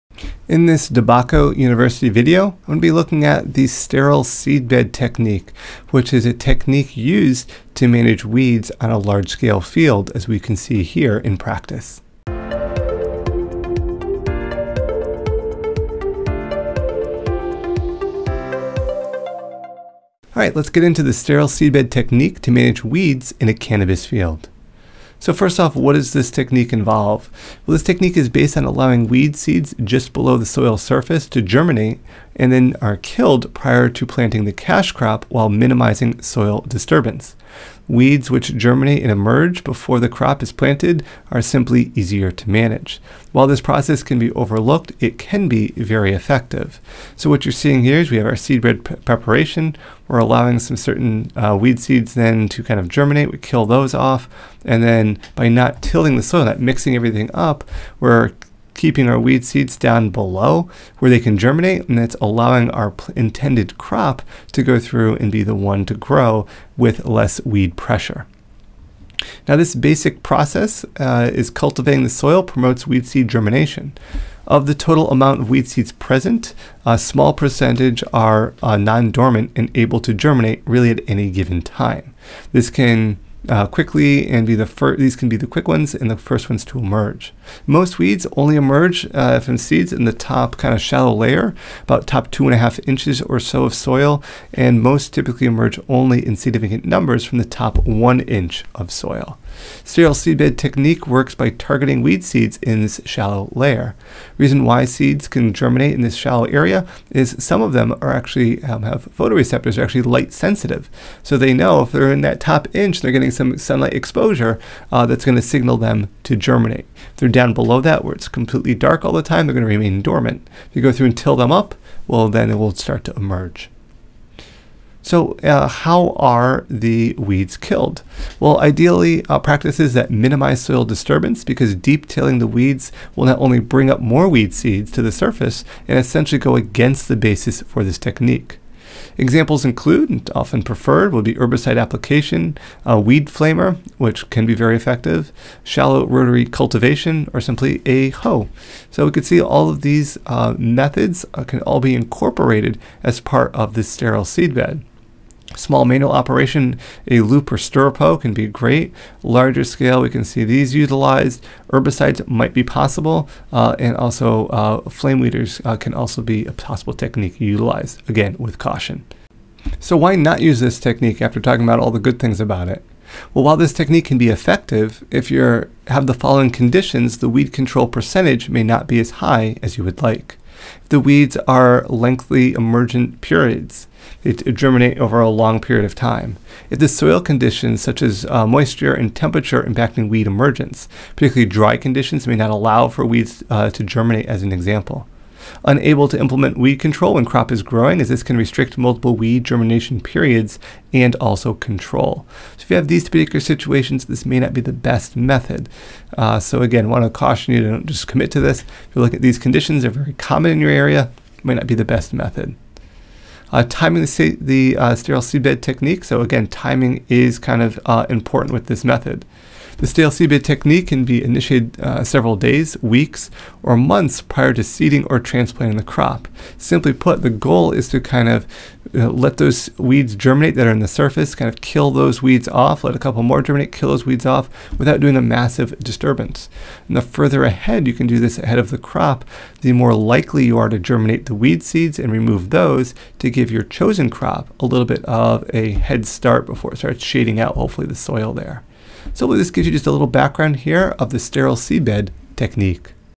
Stale Seedbed Technique to Manage Weeds in a Cannabis Field
Professor DeBacco

Stale Seedbed Technique
This technique is based on allowing weed seeds just below the soil surface to germinate and then are killed prior to planting the cash crop while minimizing soil disturbances.
Weeds which germinate and emerge before the crop is planted are easier to manage.
While this process can be overlooked it can be effective.

Basic Process
Cultivating soil promotes weed seed germination
Of the total amount of weed seeds present a small percentage is non-dormant and able to germinate at any given time
*This can quickly and be the first to emerge
Most weeds only emerge from seeds in the shallow layer (top 2.5 inches) of the soil and most typically emerge only in significant numbers from the top one inch of the soil.
Stale seedbed technique works by targeting weed seeds in this shallow layer of soil.

How Are the Weeds Killed?
Ideally practices that minimize soil disturbance, because deep tilling the weeds in will only bring up more weed seeds to the surface and essentially go against the basis for this technique.
Examples include…
Preferred…
Herbicide application
Weed flamer
Also can be effective…
Shallow rotary cultivation
Hoe

Why Not Use This Technique?
While this technique can be effective if you have the following conditions the weed control percentage may not be as high as you would like…
Weeds with lengthy emergence periods
Soil conditions such as moisture and temperature impacting weed emergence.
Dry conditions may not allow for weeds to germinate
Unable to implement weed control when crop is growing, this can restrict multiple weed germination periods/control.

Timing the Stale Seedbed Technique
The stale seedbed technique can be initiated several days, weeks, or months prior to seeding or transplanting a crop.

*Due to the description character limit the full work cited for "Stale Seedbed Technique to Manage Weeds in a Cannabis Field" can be viewed